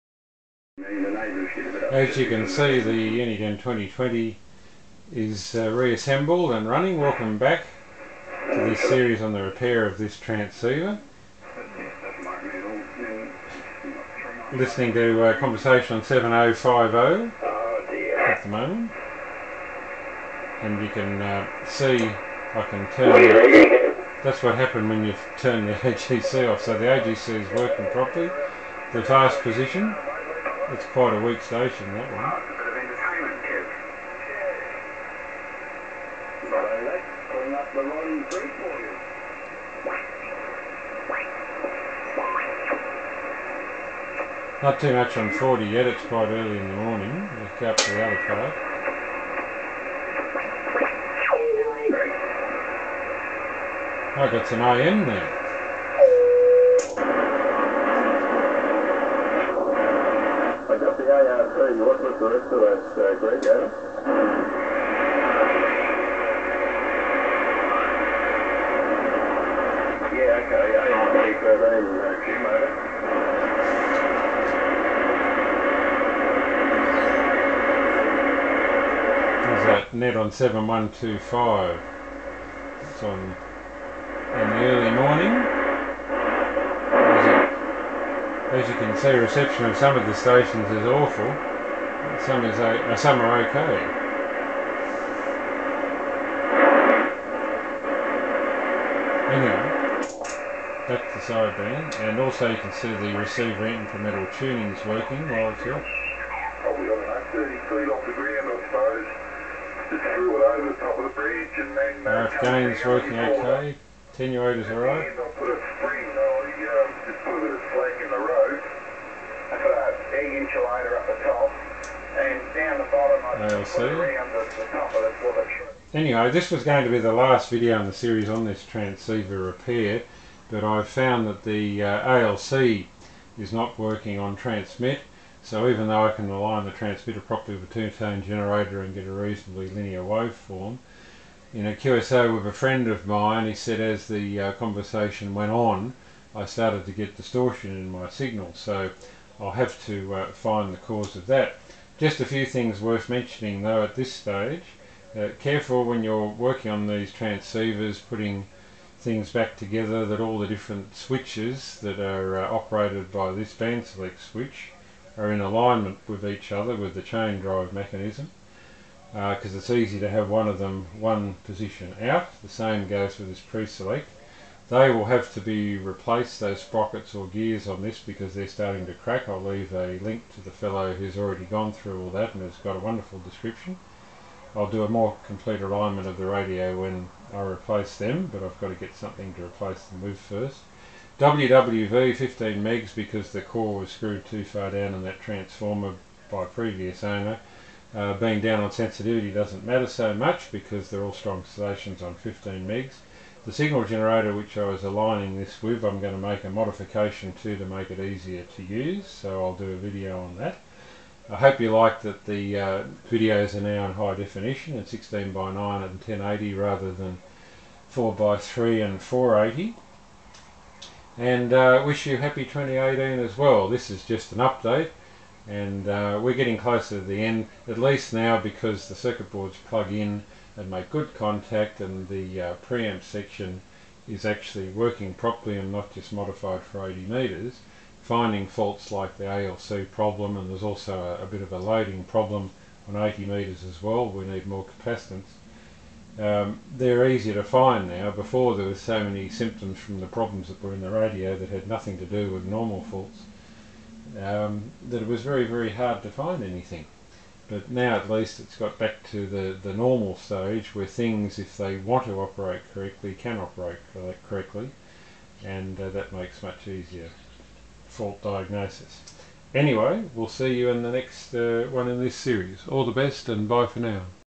Uniden 2020 Repair Part 7 of 11
A quick demonstration of the transceiver after the repairs to date with another fault to be fixed found!

More information on the Uniden/Tempo 2020 Amateur Transceiver, including replacing the sprockets, can be found at...